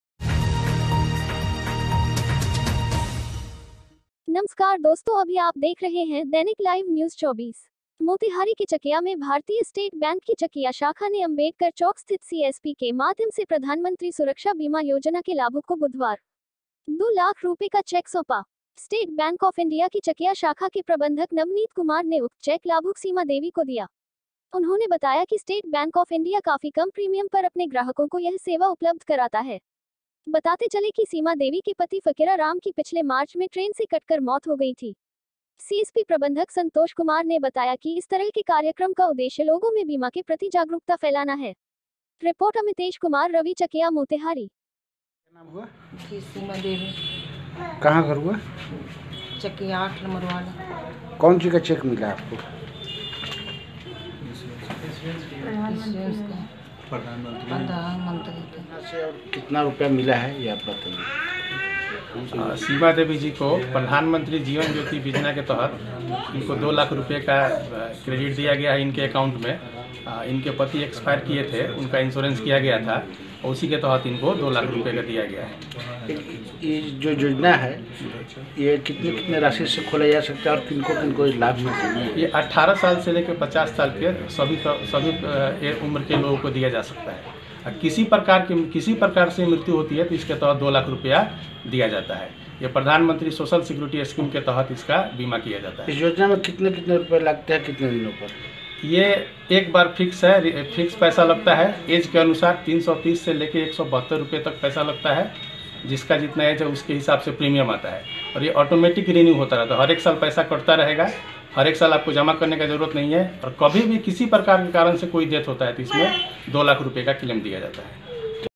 मोतिहारी के चकिया में भारतीय स्टेट बैंक की चकिया शाखा ने अंबेडकर चौक स्थित सीएसपी के माध्यम से
CHAKIA:___REPORTER :___AMITESH KUMAR RAVI MOBILE:___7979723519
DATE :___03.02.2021
SLUG :_______ लाभुक को सौंपा दो लाख का चेक।
ANCHOR :______मोतिहारी के चकिया में भारतीय स्टेट बैंक की चकिया शाखा ने अंबेडकर चौक स्थित सीएसपी के माध्यम से प्रधानमंत्री सुरक्षा बीमा योजना के लाभुक को बुधवार दो लाख रूपए का चेक सौंपा।स्टेट बैंक ऑफ इंडिया की चकिया शाखा के प्रबंधक नवनीत कुमार ने उक्त चेक लाभुक सीमा देवी को दिया।उन्होने बताया कि स्टेट बैंक ऑफ इंडिया काफी कम प्रीमियम पर अपने ग्राहकों को यह सेवा उपलब्ध कराता है ।बताते चले की सीमा देवी के पति फकिरा राम की पिछले  मार्च मे ट्रेन से कटकर मौत हो गई थी।सीएसपी प्रबंधक संतोष कुमार ने बताया कि इस तरह के कार्यक्रम का उदेश्य लोगों मे बीमा के प्रति जागरूकता फैलाना है।

बाईट :- SBI प्रबंधक चकिया नवनीत कुमार

बाईट :- लाभुक सीमा देवी